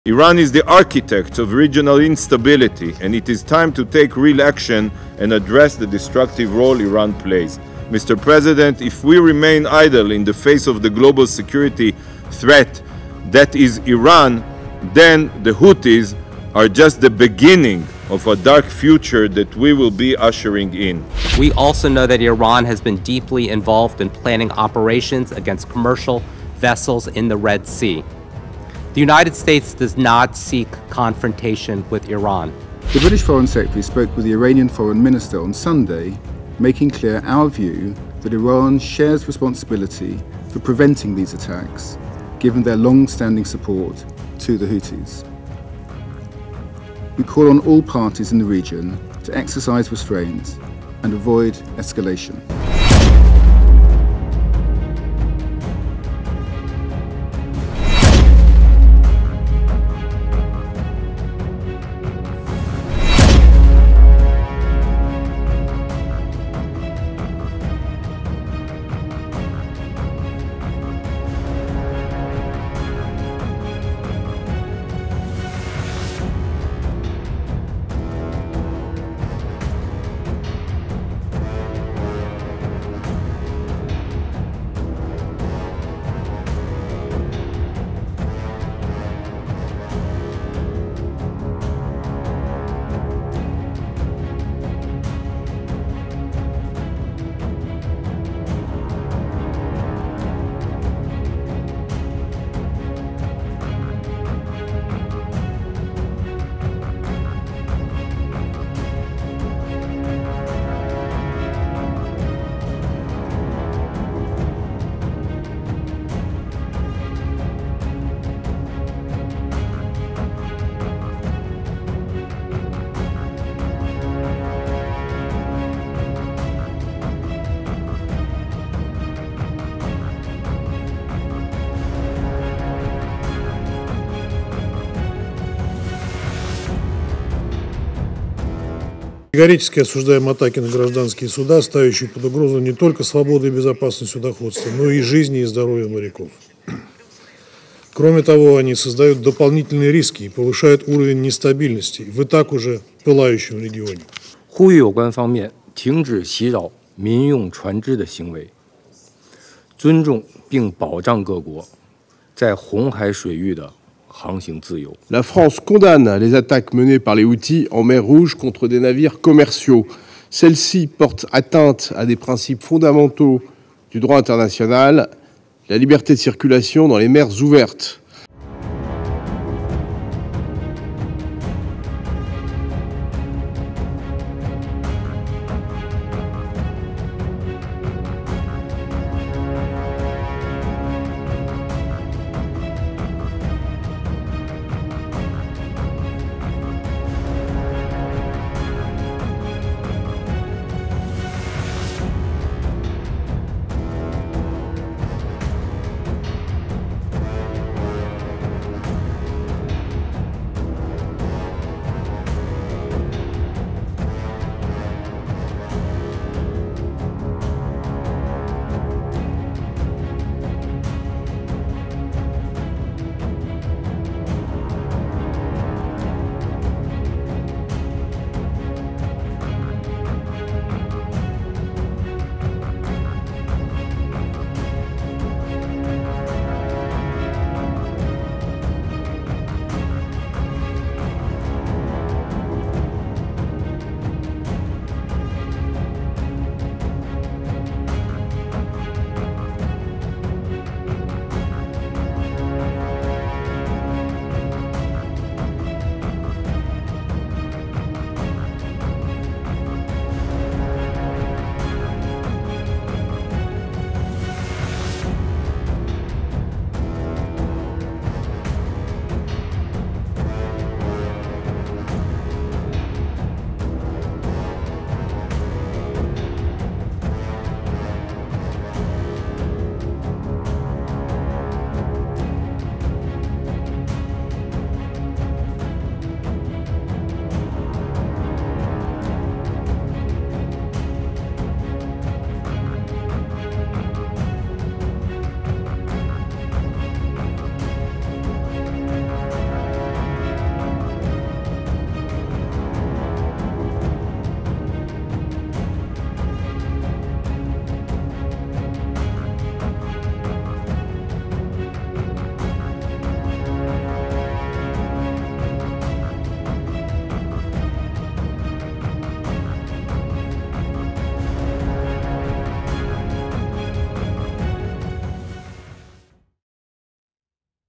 Houthi Attacks In Red Sea 'Spook' Putin's Ally China | Israel, NATO Nations Fume At Iran
Russia's ally China has opposed the attacks on vessels in the Red Sea and called for all parties involved to find a solution to ensure regional stability. The statement came as ambassadors to the UN of Israel and NATO nations blasted Iran for its alleged role in the Houthi attacks on ships since November 19. While the U.S., the UK and France named Iran and Houthis, Russian and Chinese envoys stopped short of calling them out directly. Despite the statements, the UN security council could not come to any conclusion on how to stop the assault by the Houthi rebels in the Red Sea and the Gulf of Eden that has affected global trade in the major shipping route. Watch for more details.